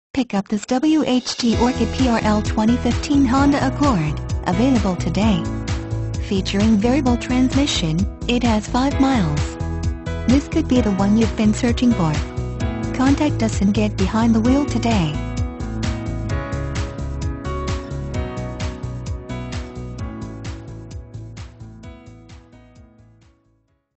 2015 HONDA ACCORD Oakland,Hayward,Alameda,San Lea 37322
2015 HONDA ACCORD Oakland,Hayward,Alameda,San Lea
Stock# 37322

Pick up this WHT ORCHID PRL 2015 HONDA ACCORD , available today. Featuring Variable transmission, it has 5 miles. This could be the one you've been searching for.